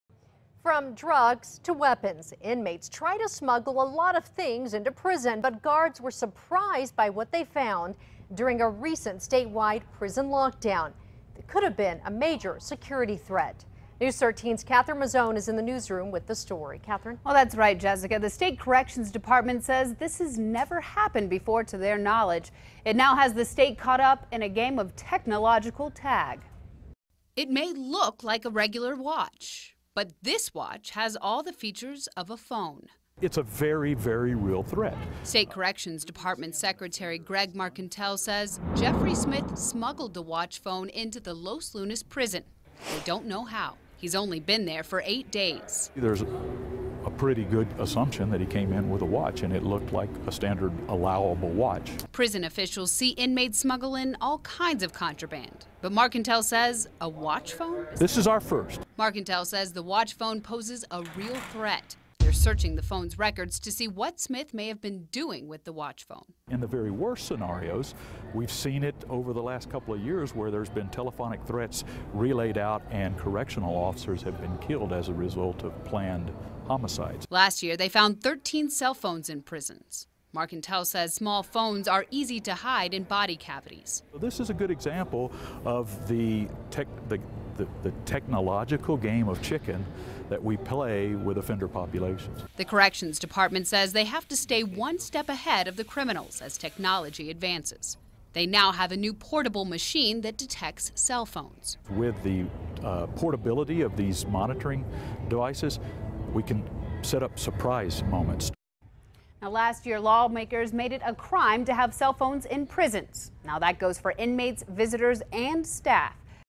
Dept. of Corrections finds watch phone in lockdown search
A statewide prison lockdown turns up a major security threat. And it's nothing like this corrections department has ever seen. That threat turned out to be a cell phone but not just any cell phone. One disguised as a watch. It has the state department of corrections caught up in a game of technological tag.